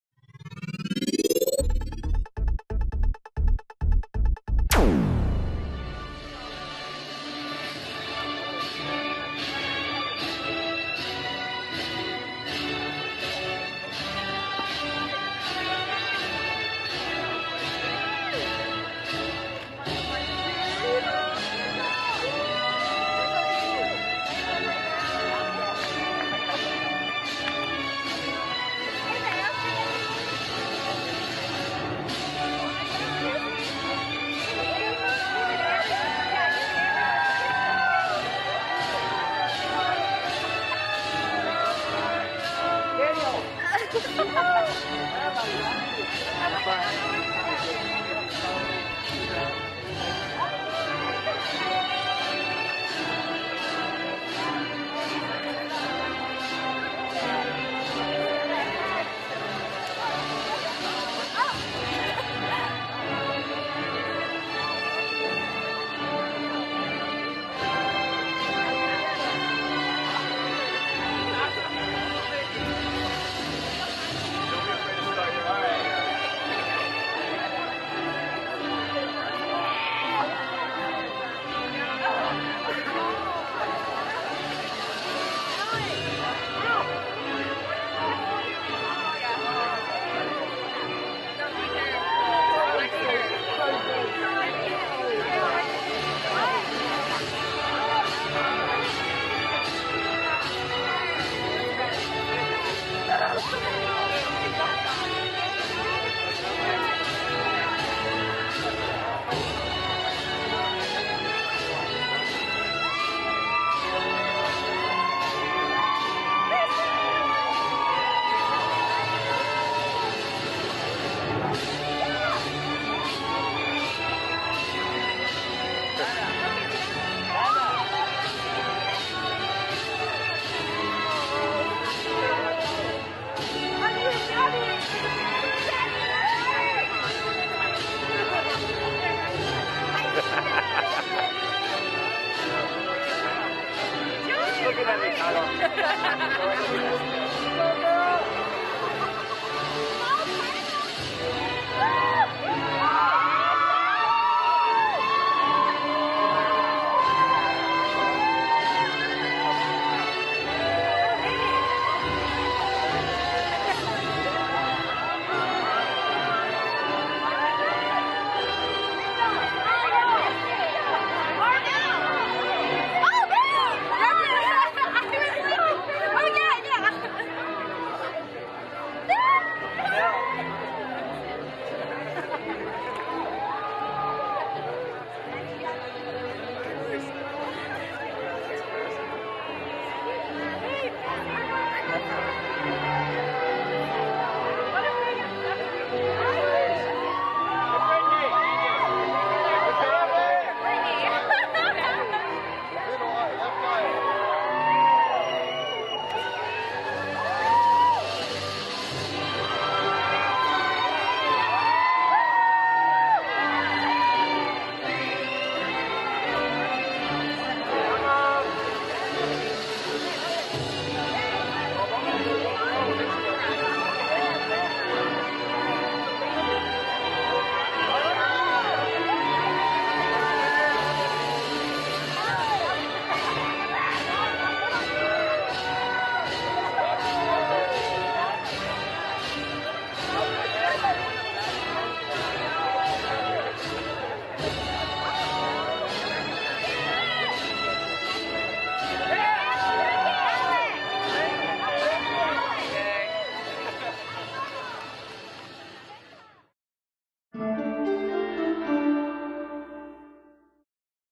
SJSU Communication Studies 2014 Graduation Procession
Here is a fixed camera view of the graduating seniors from the San Jose State University Communication Studies department for the class of 2014. If you see yourself or a friend in the video, leave a shout out in the comments section with the time code where you or your friend are shown. Congratulations to you all, the best is yet to come!